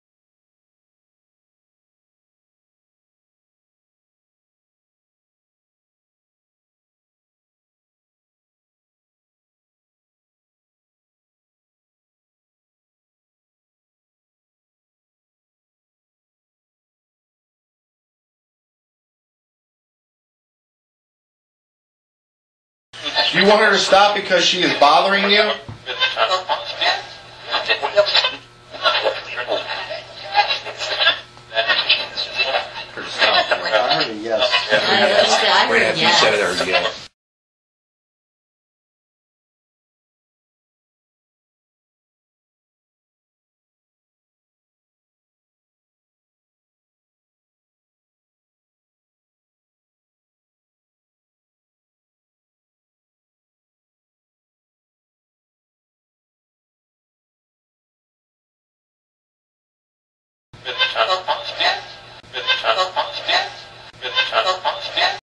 femyes.wmv
From our New York investigation a response to a question during the minibox session.